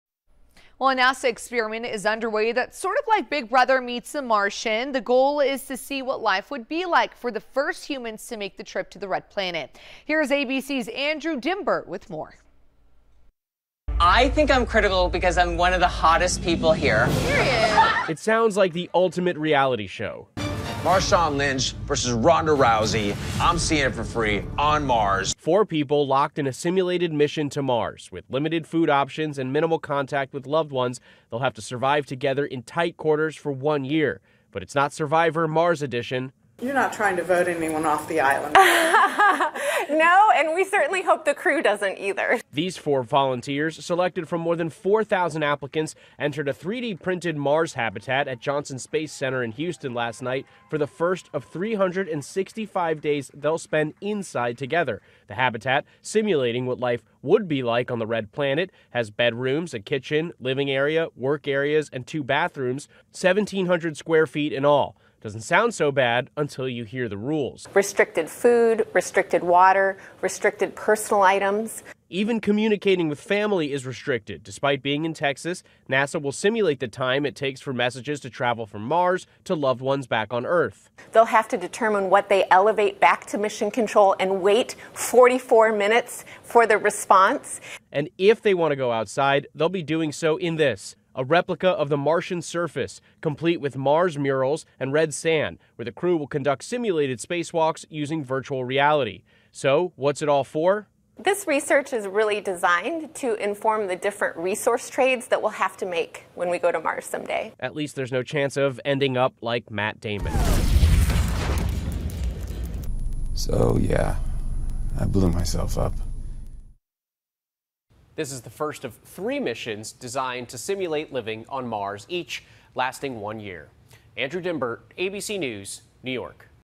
NASA experiment replicates life on Mars
Four people were locked in a simulated mission to Mars.